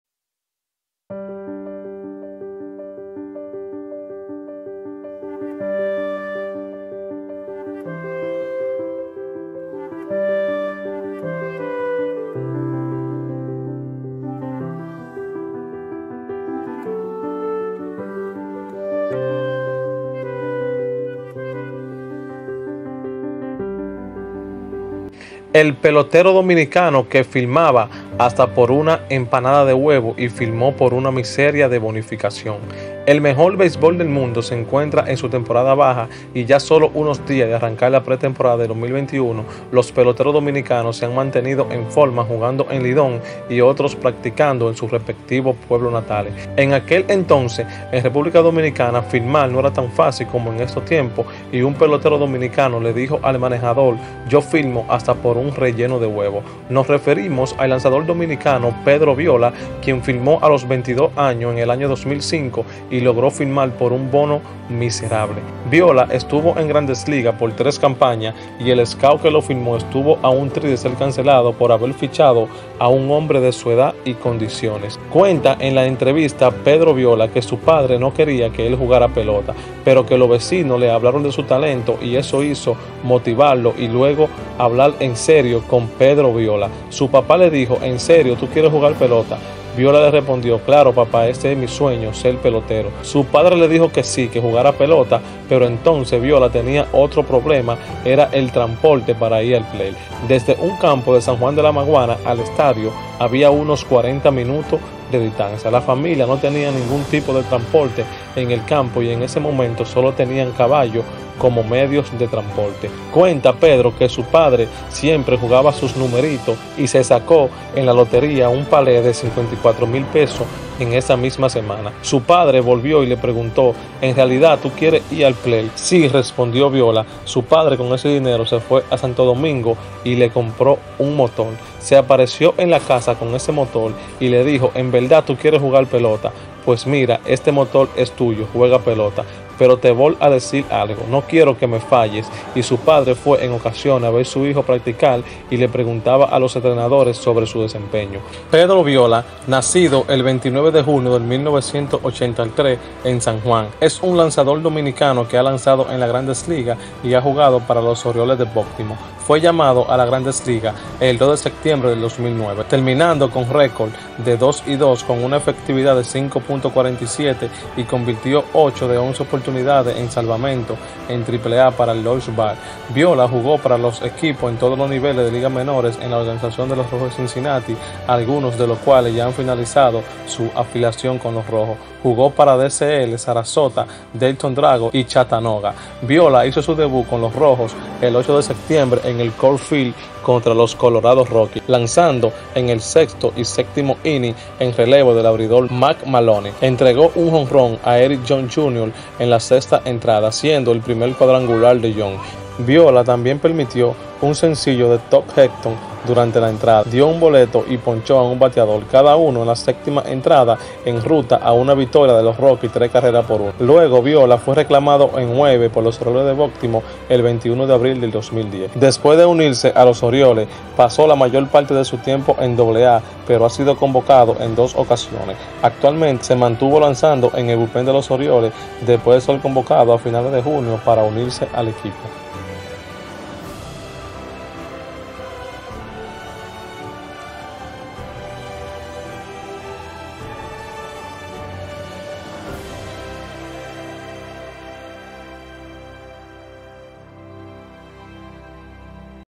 El Pelotero Que Firmaba Hasta Gratis Y Llego A Mlb
Pedro Viola (nacido el 29 de junio de 1983 en San Juan) es un lanzador dominicano de Grandes Ligas